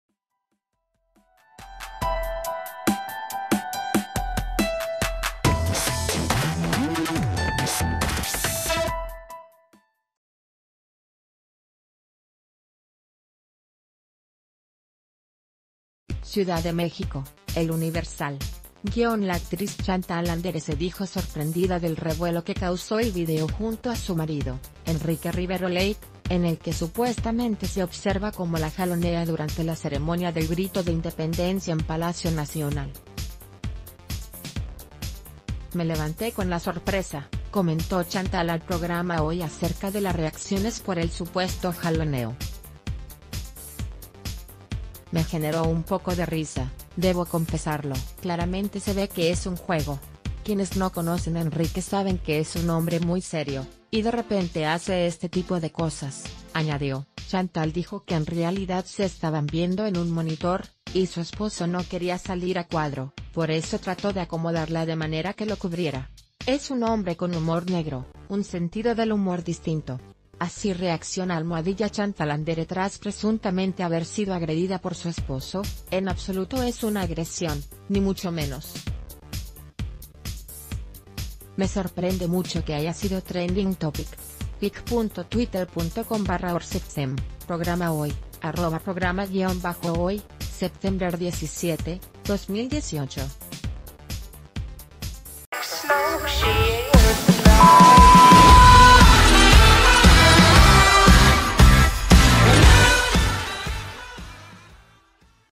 Chantal Andere se ríe del supuesto jaloneo en Palacio Nacional - El Diario de Yucatán
CIUDAD DE MÉXICO (EL UNIVERSAL).- La actriz Chantal Andere se dijo sorprendida del revuelo que causó el video junto a su marido, Enrique Rivero Lake, en el que supuestamente se observa cómo la jalonea durante la ceremonia del Grito de Independencia en Palacio Nacional. “Me levanté con la sorpresa“, ...